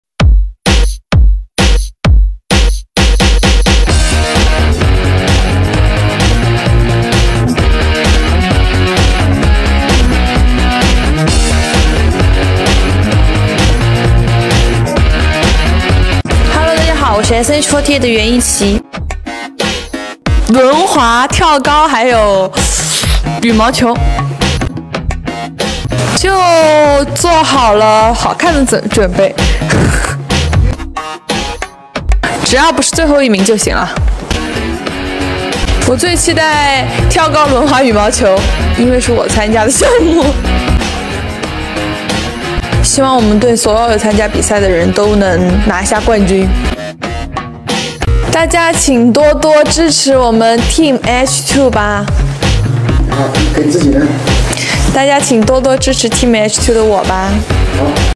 [ YUAN YIQI ] Team HII pre-game interview online | SNH48 GROUP 4th Idol Games ENG SUB
20231109 SNH48 Weibo

# SNH48偶像运动会
“集合，女孩！”SNH48 GROUP第四届偶像运动会
Team HII赛前小采访上线
同心协力 创造属于我们的奇迹
11月11日 丨 14:00 丨杭州体育馆

# SNH48 idol sports meet
"Assemble, girls!" SNH48 GROUP 4th Idol Games
Team HII pre-game interview online
Work together to create our miracles
November 11 | 14:00 | Hangzhou Stadium